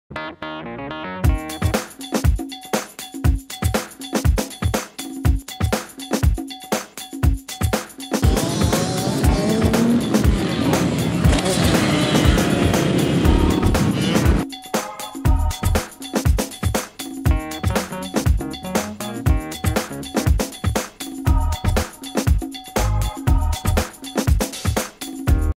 KX 65 Bottom Out, Jump
I bottomed out on my 2008 KX 65 on a track jumping